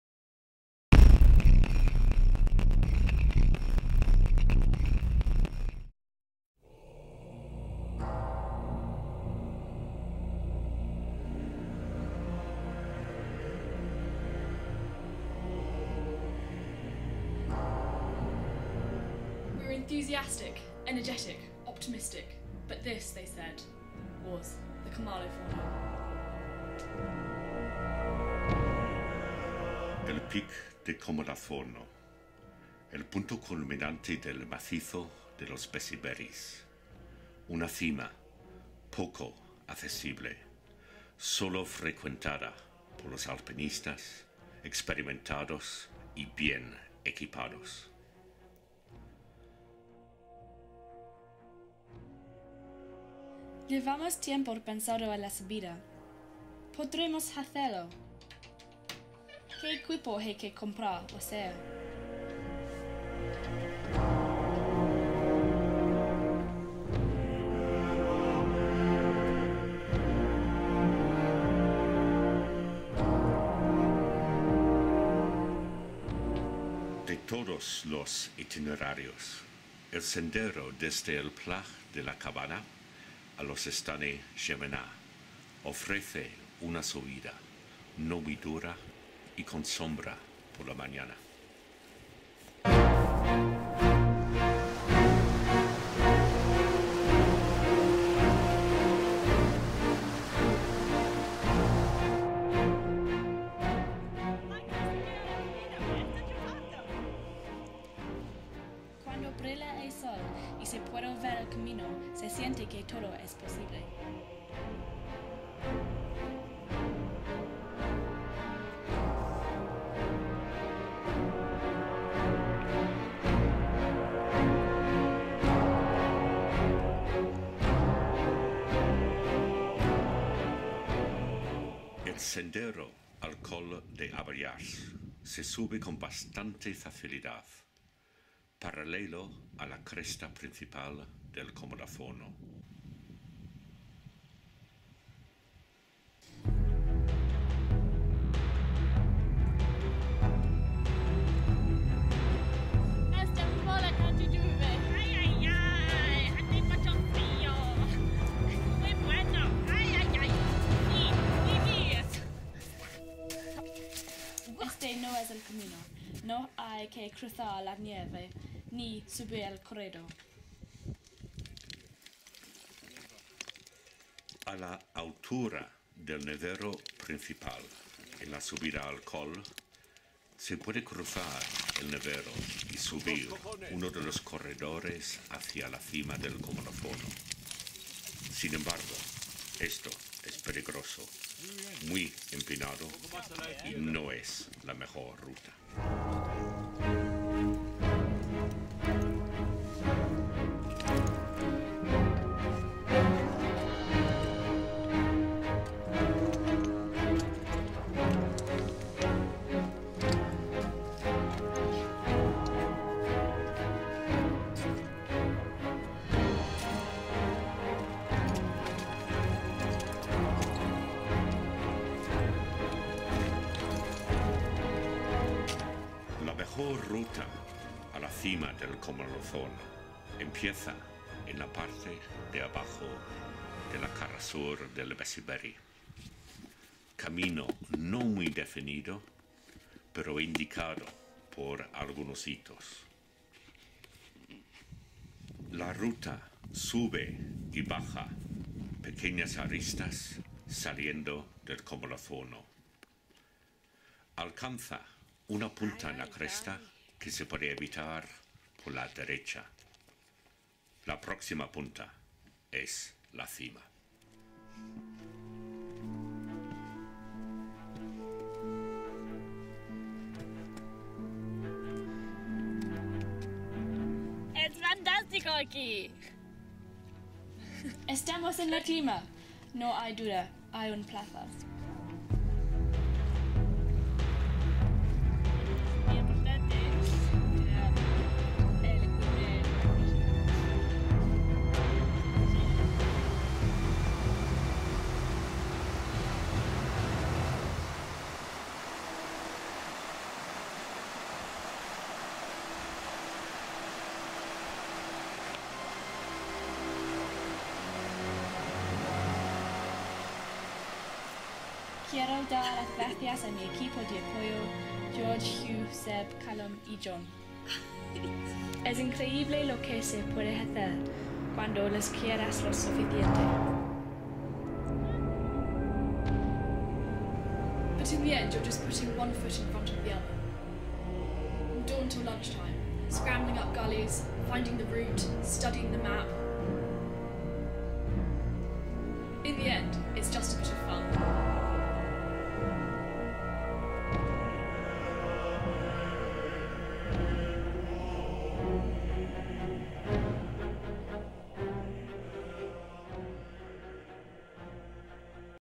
Camino de Comaloforno
How to climb the Comaloforno, girl power style, with expert Spanish commentary